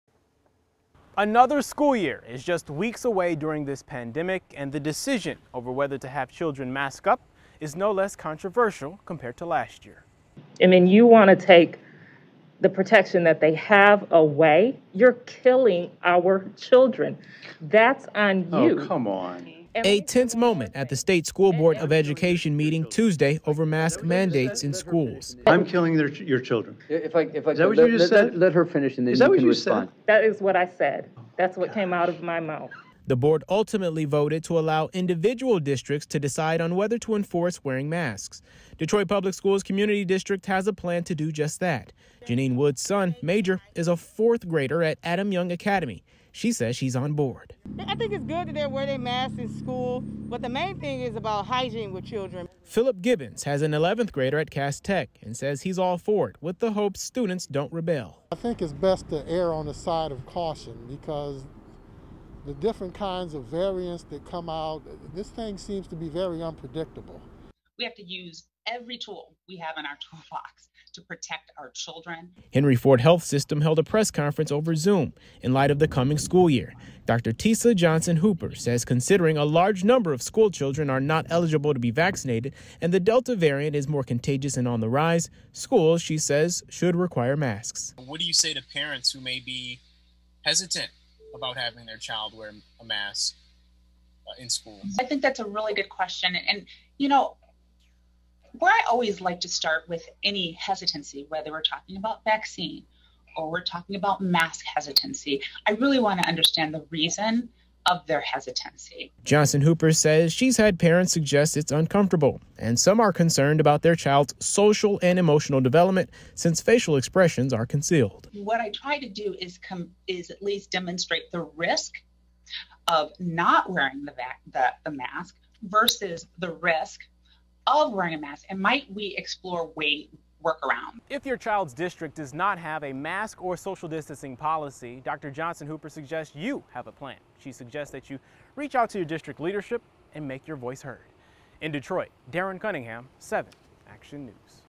Michigan Department of Education leaving the decision for masks up to the districts
School districts are faced with the decision of whether to enforce mask mandates. Unlike last year, it's not a decision made at the state level.